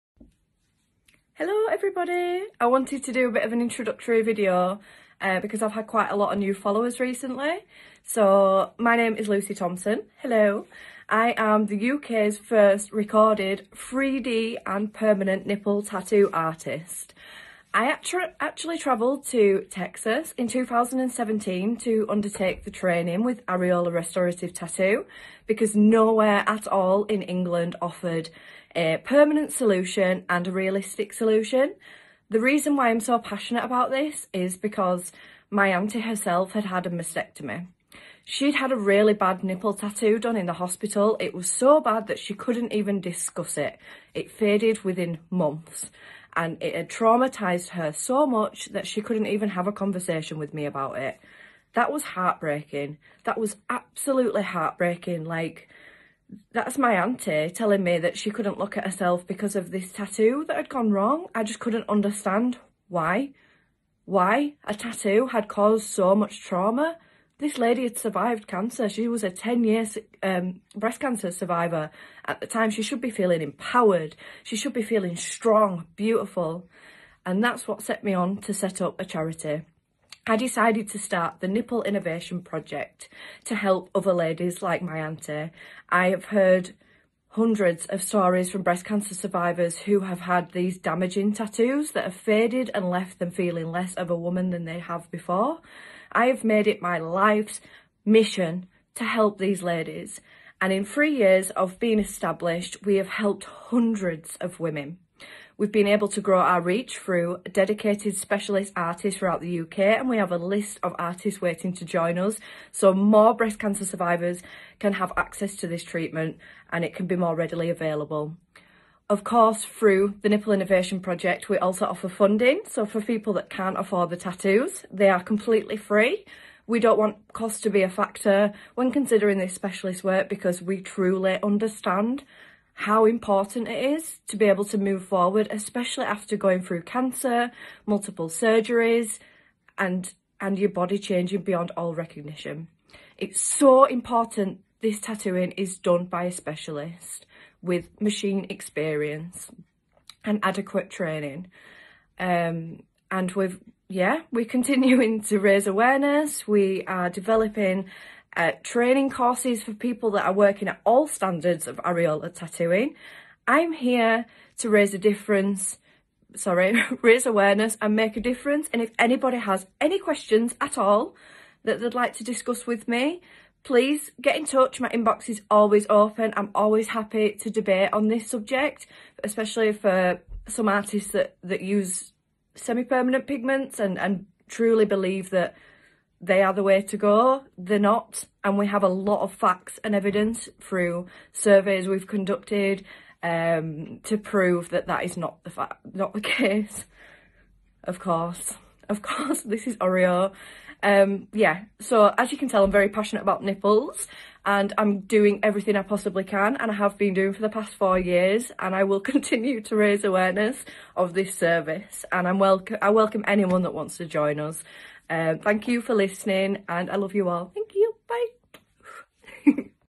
Lucy Thompson 3D Nipple & Mastectomy Tattoo Artist
A little bit about me, why i do what i do and what amazing work we do through the UKs breast cancer mastectomy tattoo charity The Nipple Innovation Project or on instagram @nipple_project